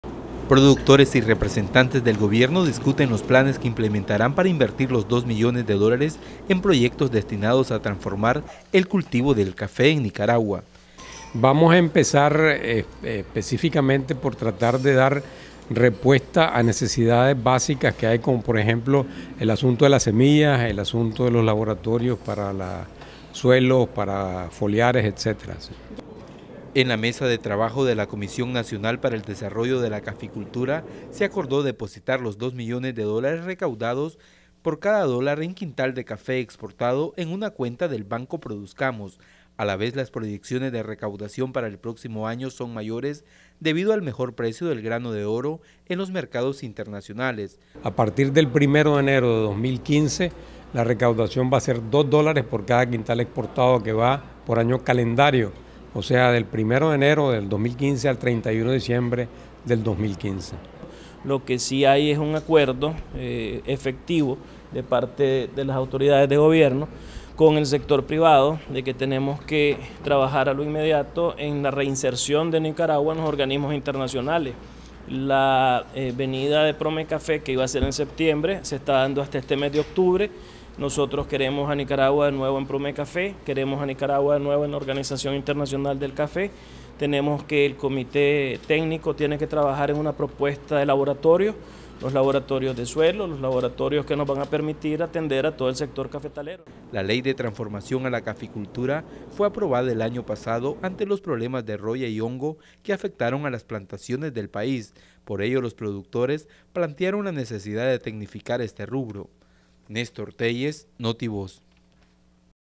Notivos: Inician gestiones para transformar la caficultura en Nicaragua 03/10/14
La Comisión Nacional  Para el Desarrollo y Transformación de la Caficultura logró recaudar este año dos millones de dólares por cada quintal del grano de oro exportado, este monto será invertido en planes de  desarrollo de este sector.